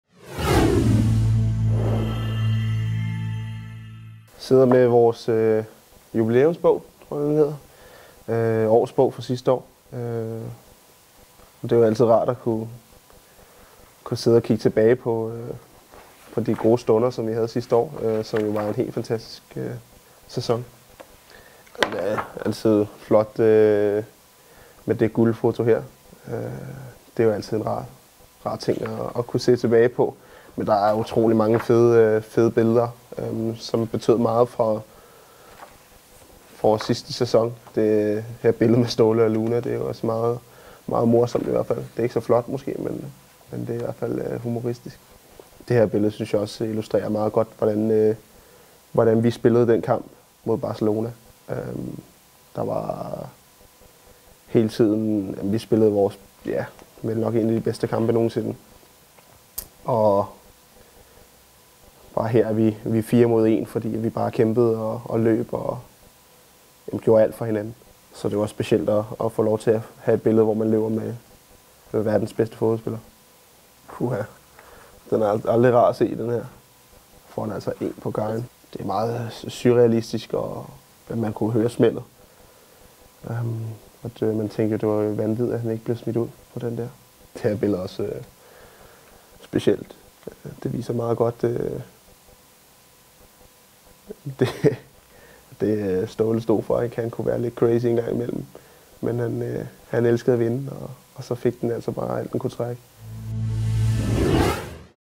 Zanka kigger tilbage
"Årsbogen F.C. København 2010/2011" kigger tilbage på den fantastiske sæson, hvor F.C. København, som den første klub i dansk fodboldhistorie, kvalificerede sig til 1/8-finalerne i UEFA Champions League, ligesom mesterskabet blev vundet i ganske suveræn stil. Selvom F.C. København altid kigger fremad, er det altid rart at se tilbage og mindes en historisk sæson. Vi bad Mathias Zanka Jørgensen om at kigge bogen i gennem og sætte ord på de minder og billeder fra sæsonen 2010/2011, som han fandt. Du kan se Zankas nostalgiske tilbageblik på FCK TV her...